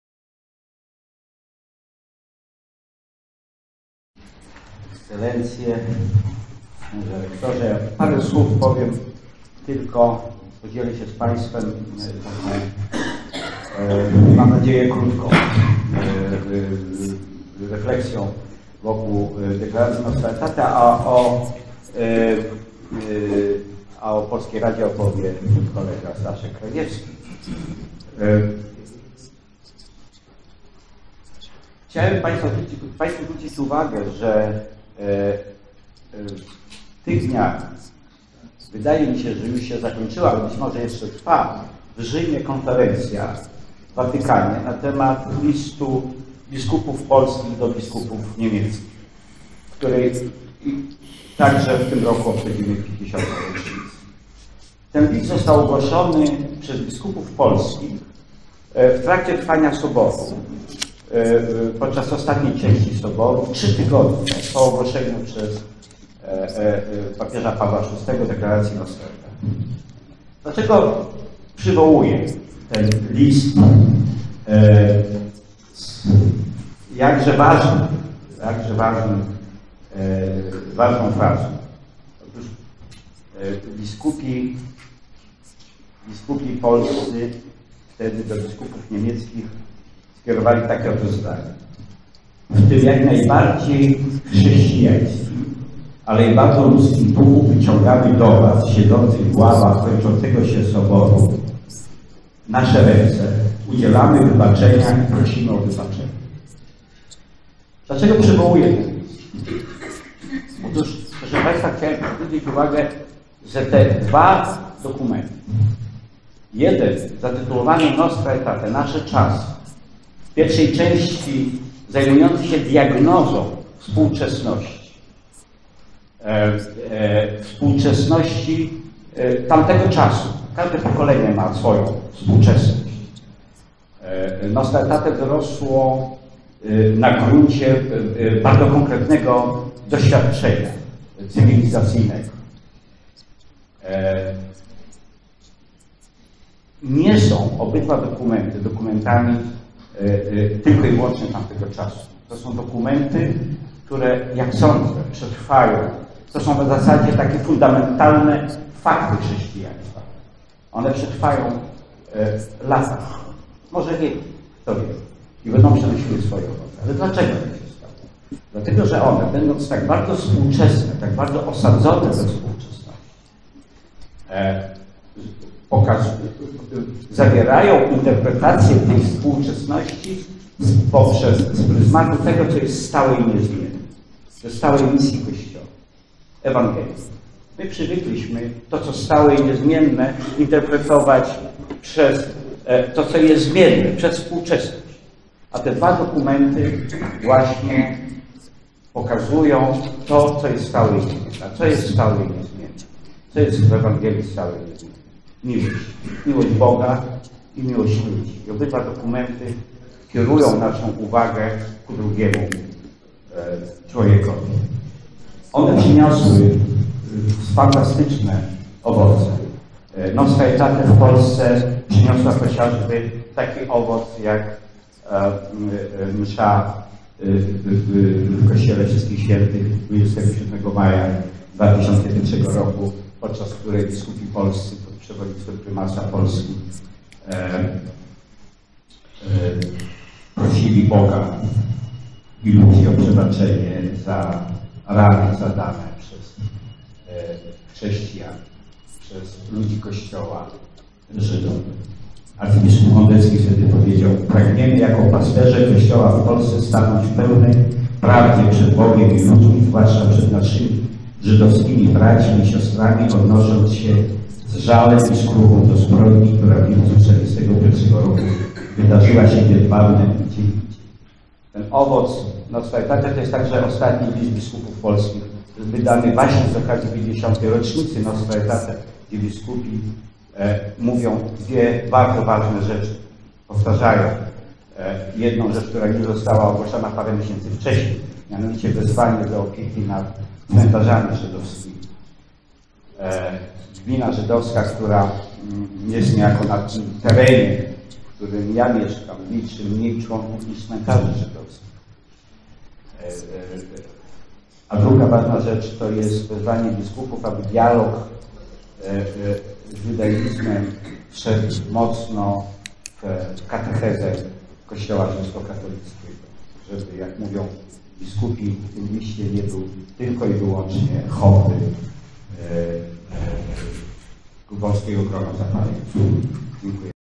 Dwa głosy - żydowski i chrześcijański o dokumencie "Nostra aetate"
Wprowadzenie Bogdana Białka na temat  dokumentu "Nostra aetate".
Sympozjum zorganizowane przez Polską Radę Chrześcijan i Żydów jest pierwszą częścią międzynarodowej konferencji naukowej „Deklaracja Soboru Watykańskiego II o stosunku Kościoła do religii niechrześcijańskich „Nostra aetate” - pięćdziesiąt lat po (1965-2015)”.
W Warszawie konferencja odbyła się również pod nazwą „Czas jubileuszy - czas nowego otwarcia”. Prelegentami byli ze strony żydowskiej Naczelny Rabin Rzymu Riccardo di Segni, ze strony chrześcijańskiej - biskup Grzegorz Ryś.
Nawiązali do soborowej deklaracji oraz 25-lecia istnienia Rady.